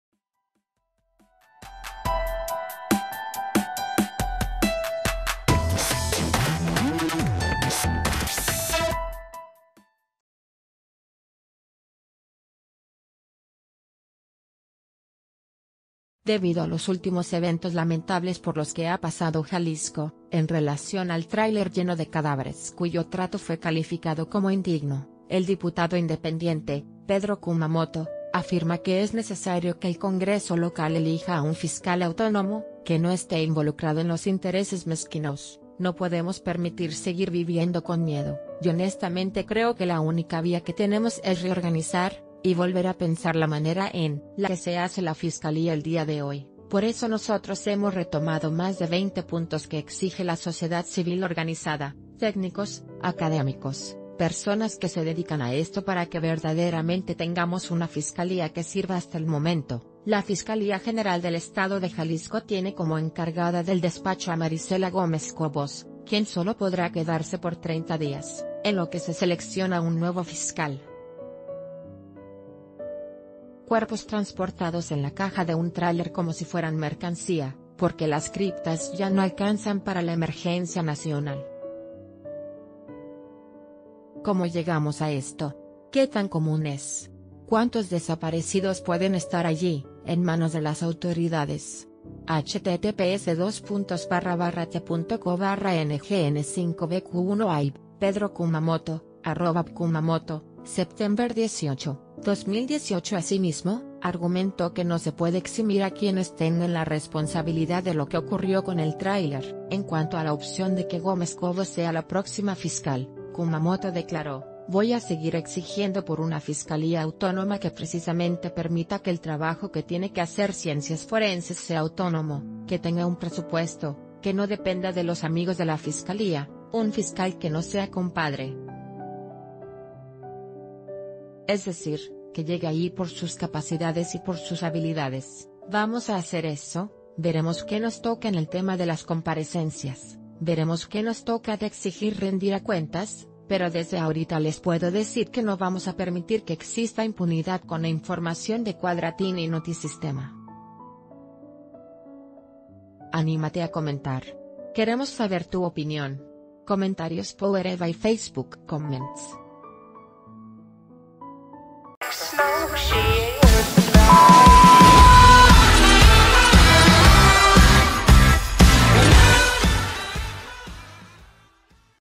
Pedro Kumamoto: urge la creación de una fiscalía autónoma
Debido a los últimos eventos lamentables por los que ha pasado Jalisco, en relación al tráiler lleno de cadáveres cuyo trato fue calificado como indigno, el diputado independiente, Pedro Kumamoto, afirma que es necesario que el Congreso local elija a un fiscal autónomo, que no esté involucrado en lo...